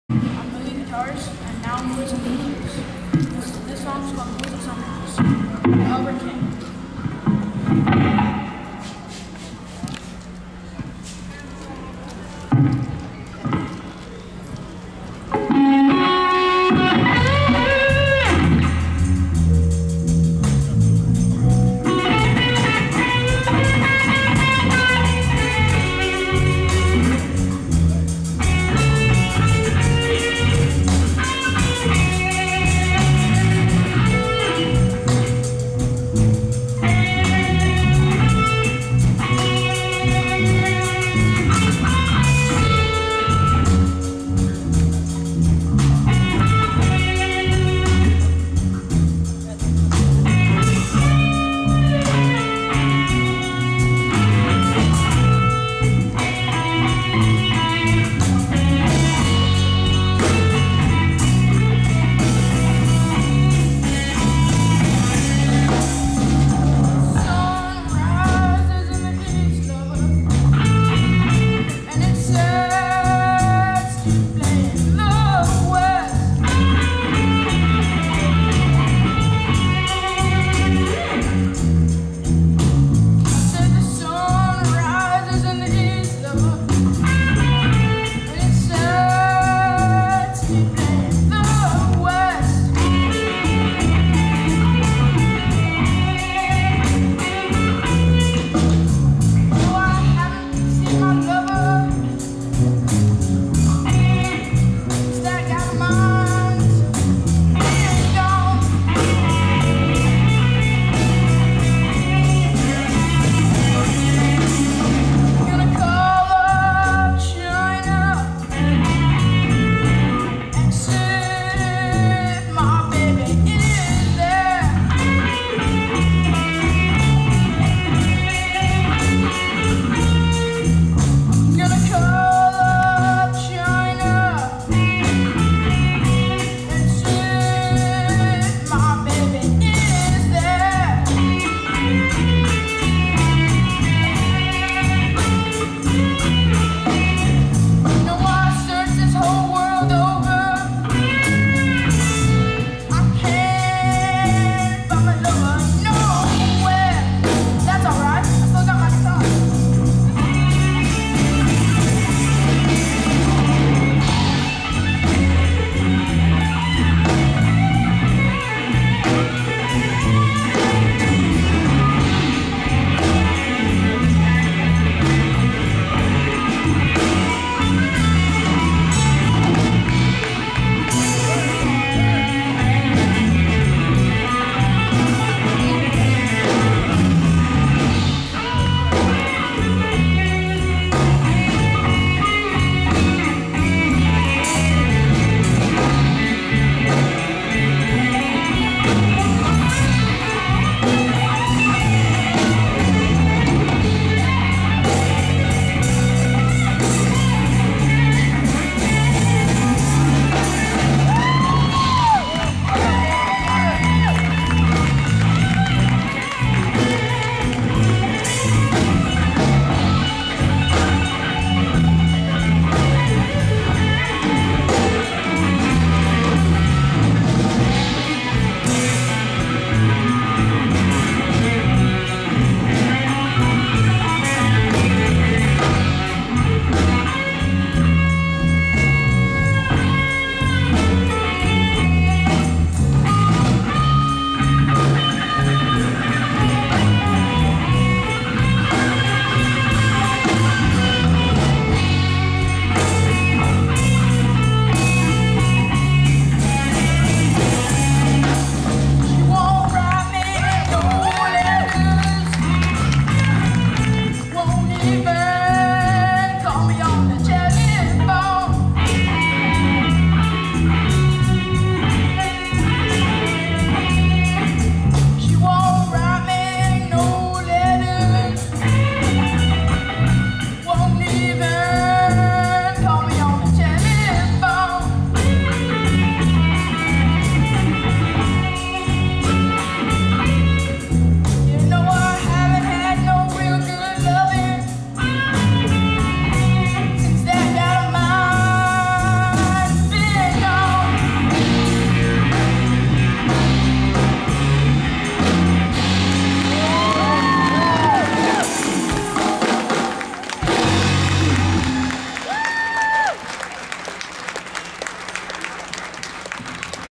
Soul of Anubis Performing Blues at sunrise
The Soul of Anubis performed at the July 10, 2011 Mafrika Music Festival in NYC